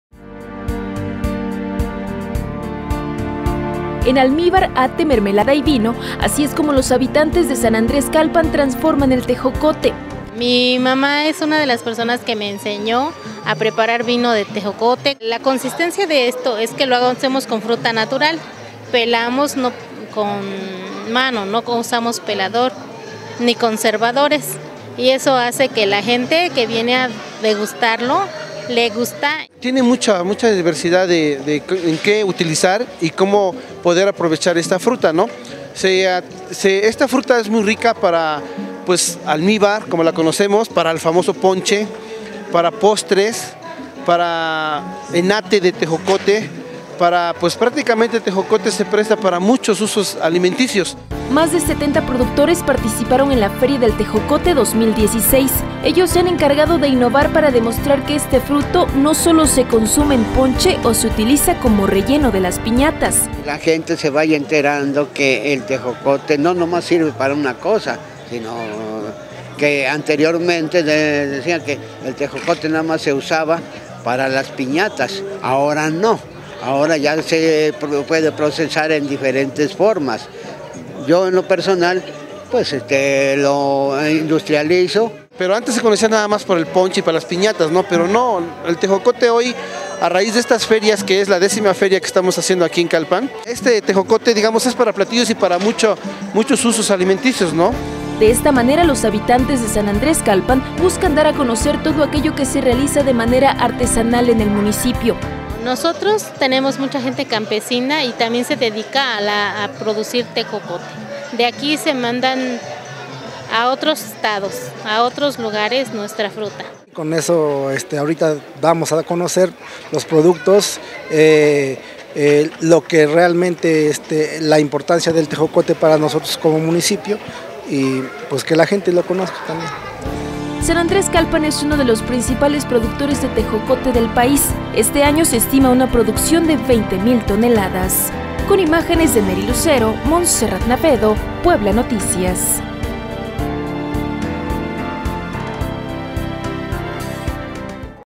Un éxito la Feria del Tejocote de San Andrés Calpán
Participaron 70 productores vendiendo productos de este fruto